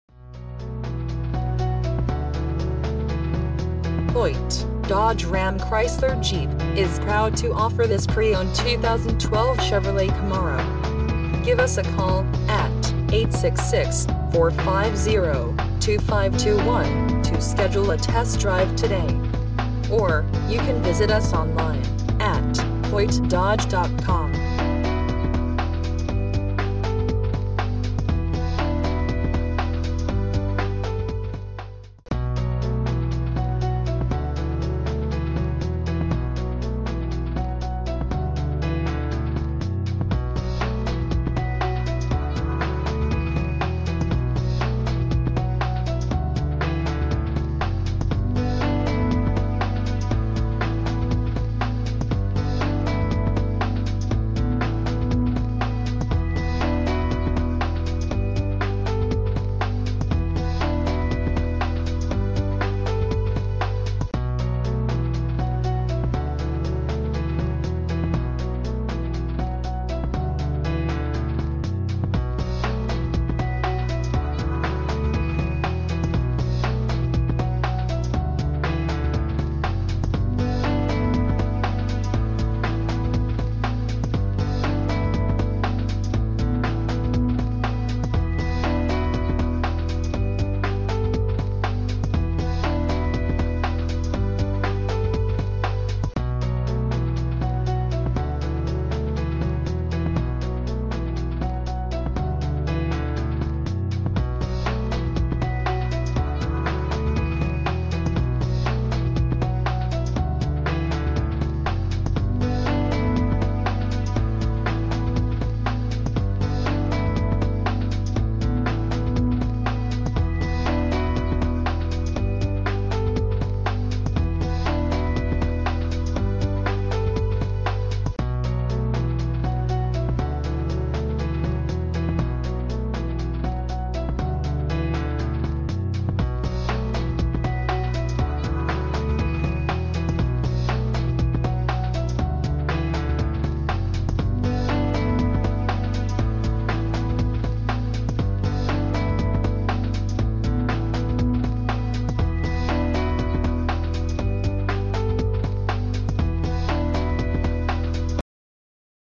Used 2012 Chevrolet Camaro 1LS Plano TX
Hoyte Dodge RAM Chrysler Jeep is proud to offer Plano TX this used 2012 Chevrolet Camaro 1LS White 3.6L V6 DGI DOHC VVT.

2300 N. HWY 75